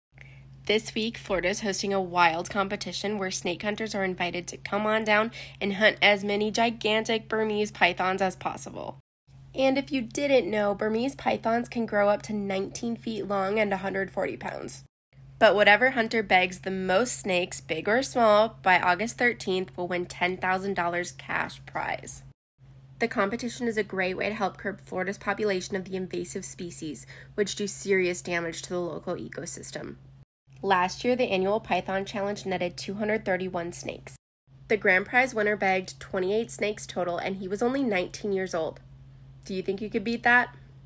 2023 Florida Python Challenge
The annual FloridaPythonChallenge takes place to help cut down the Burmese python population because there are SO many of these invasive snakes in the state. 🐍

‼️ Why are they a problem?

➡️ The massive snakes don't have many predators, but they do have a lot of prey—dozens of species crucial to the Florida ecosystem, including endangered Key Large wood rats, marsh rabbits, foxes, birds, even iguanas, some of which have become near non-existent in areas the pythons have embedded themselves.